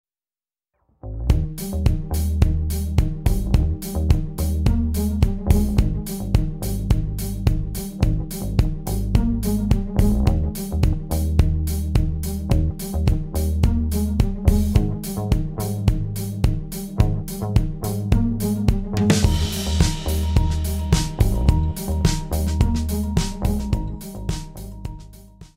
Librem 5 smartphone - Hardware Kill Switches
A quick demonstration of using the hardware kill switches on a Librem 5, privacy-focused smartphone.